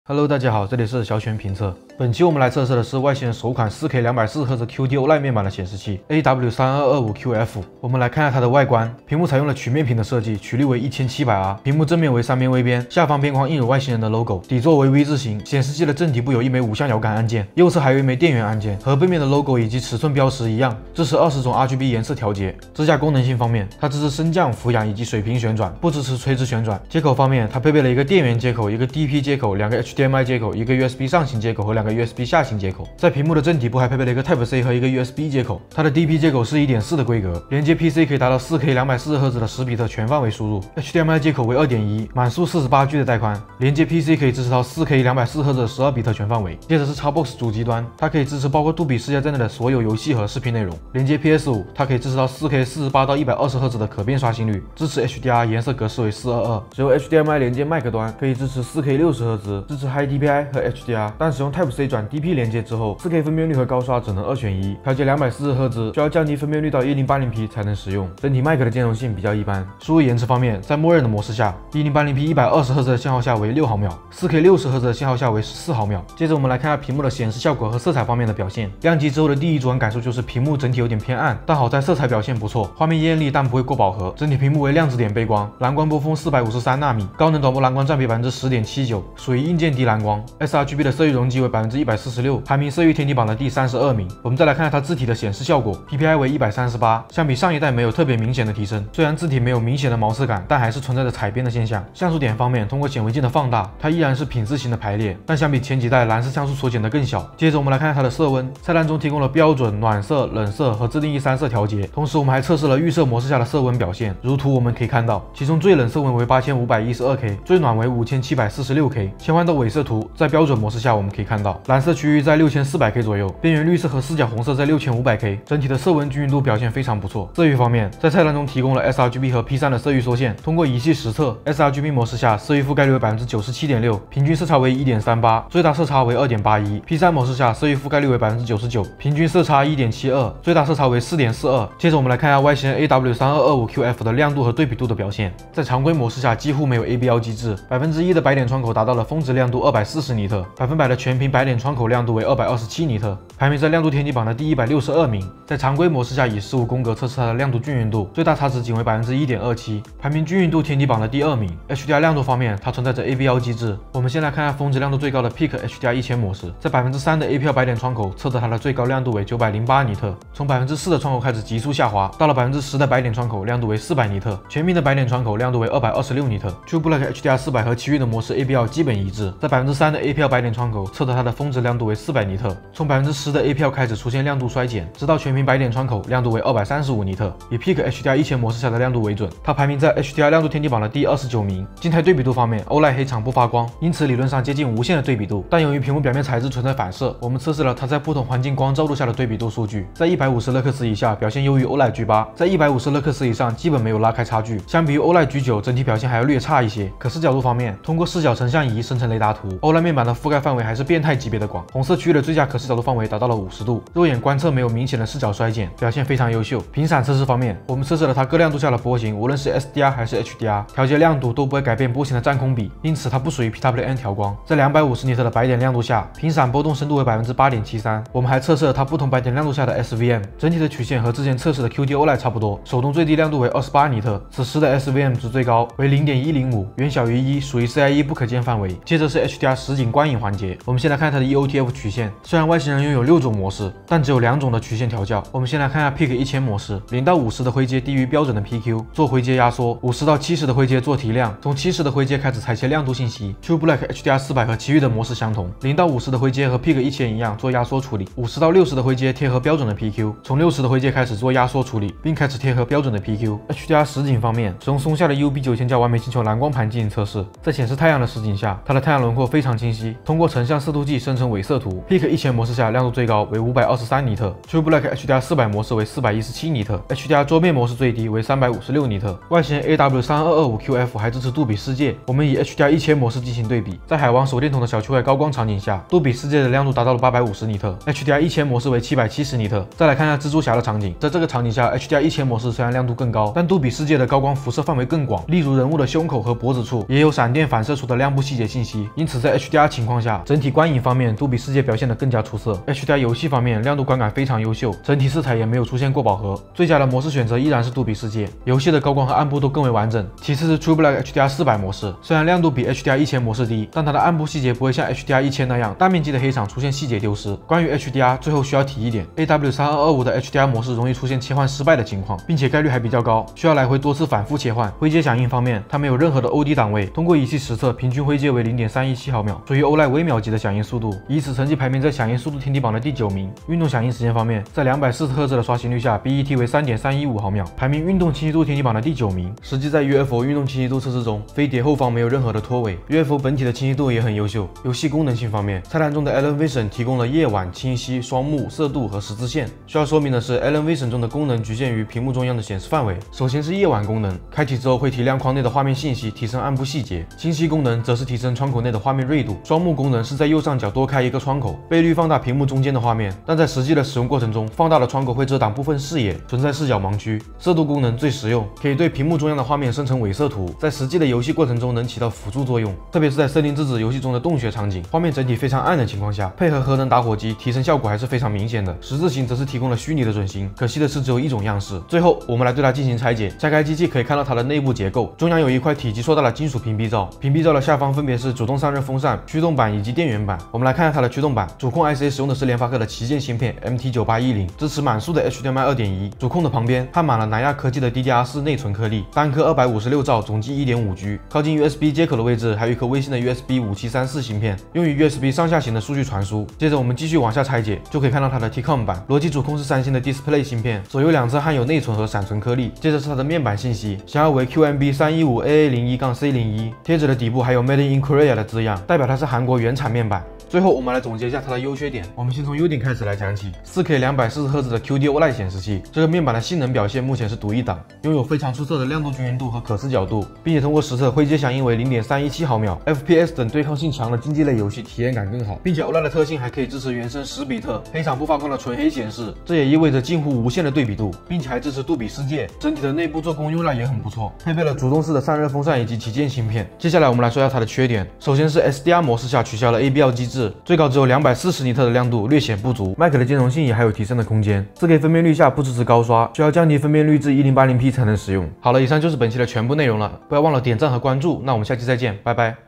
AlienWare 4K 240Hz QD-OLED Montior Review丨外星人首款4K 240Hz QD-OLED显示器 AW3225QF全面评测报告
本期我们评测的是外星人首款QD-OLED显示器AW3225QF，32英寸3840×2160 240Hz，评测内容主要针对外观设计、菜单功能、显示效果（色温、亮度、色域、均匀度、PPI、像素排列、可视角度、HDR、观影、游戏体验）接口带宽、兼容性、拆机做工用料、以及屏幕实测数据进行全面测试。